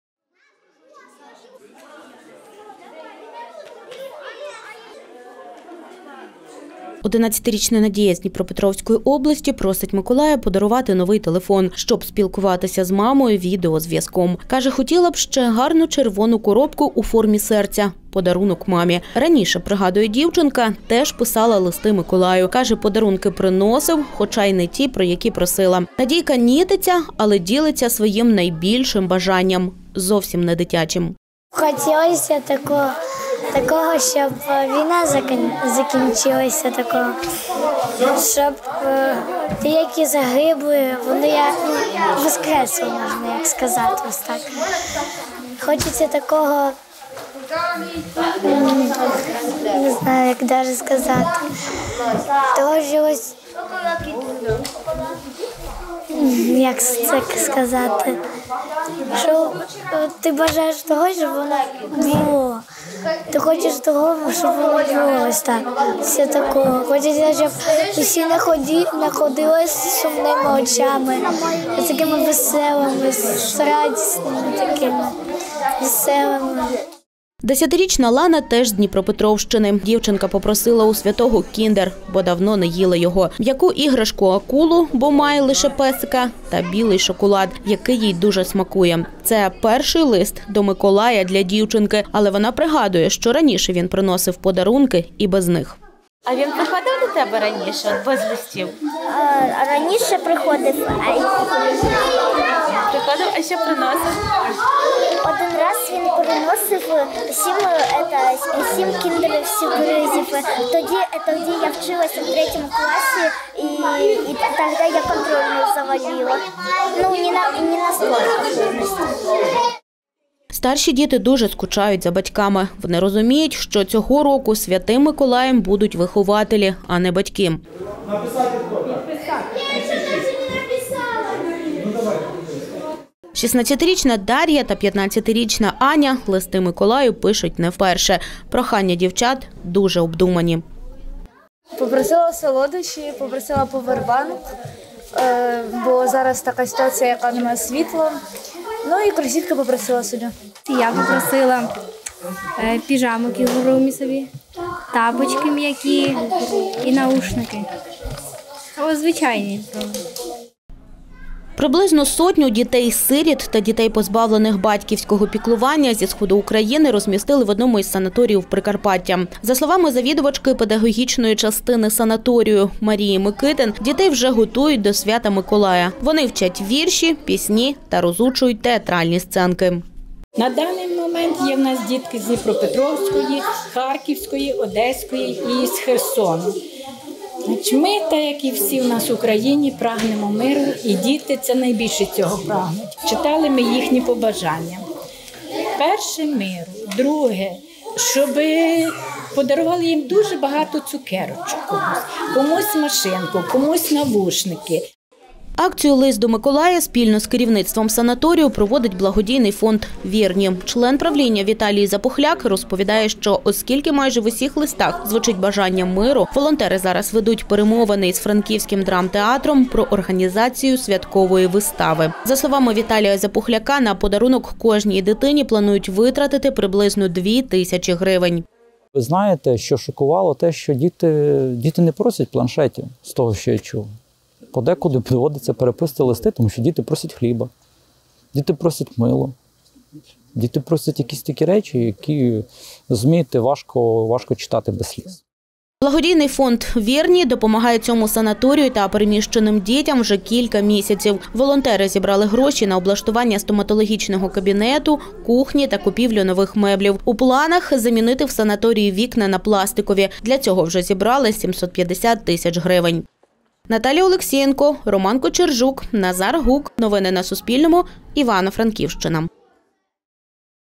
Діти-сироти зі Сходу та Півдня, які проживають на Франківщині, написали листи до Миколая
Приблизно сто дітей-сиріт та позбавлених батьківських прав зі сходу та півдня України, які зараз проживають на Івано-Франківщині, написали листи святому Миколаю. Найчастіше у своїх посланнях вони просять миру та багато солодощів.